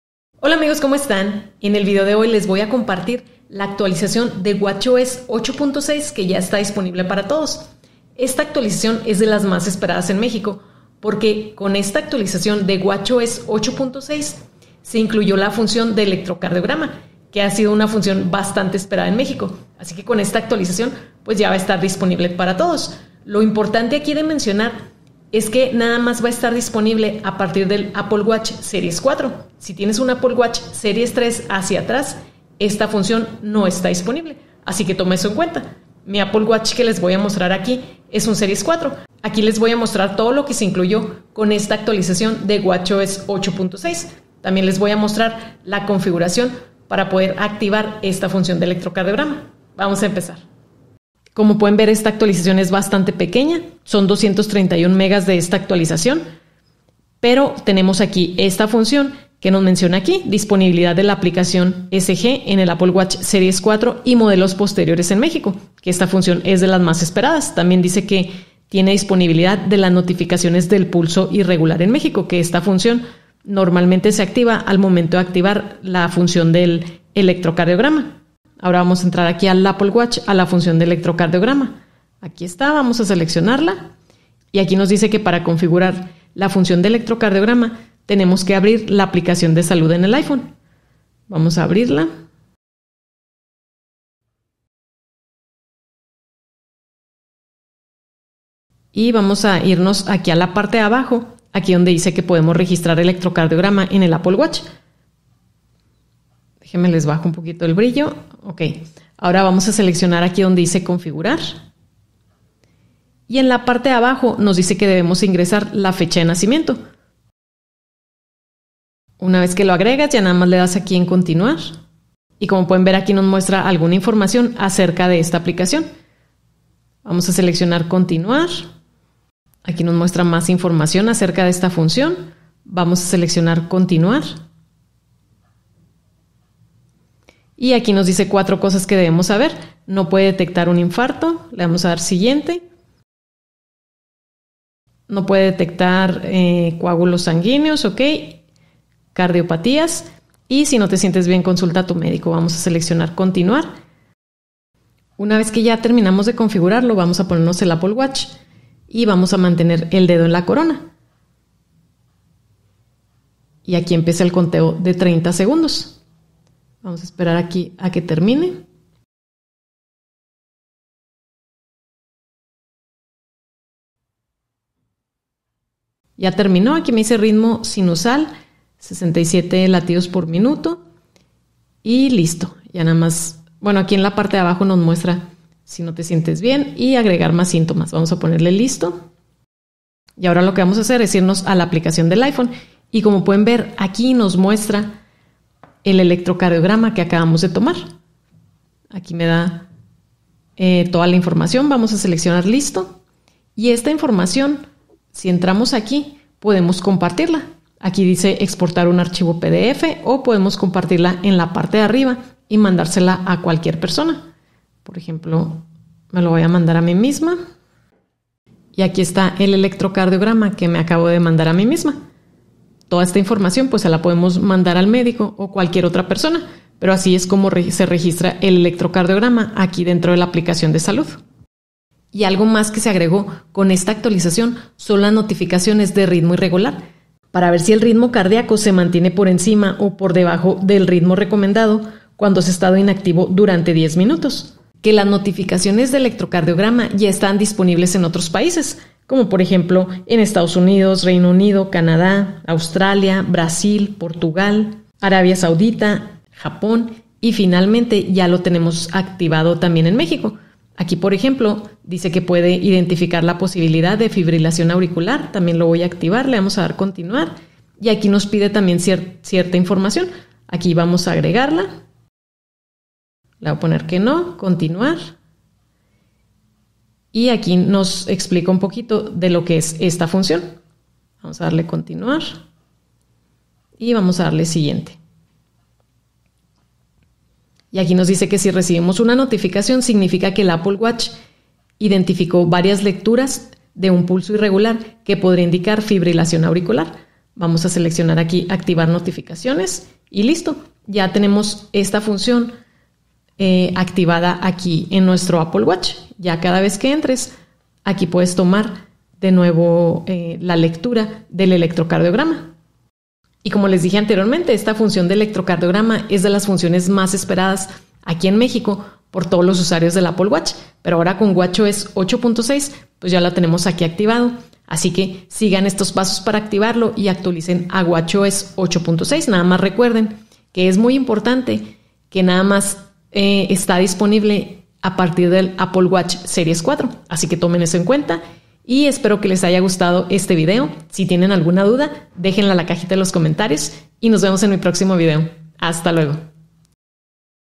WatchOS 8.6 ECG Disponible En Mexico 🇲🇽⌚️ Novedades & Configuración!
Hola amigos, ya esta disponible watchOS 8.6 con importantes cambios.

Se Descarga Muy Rápido La BATERIA De Tu iPhone? 💡SOLUCION!!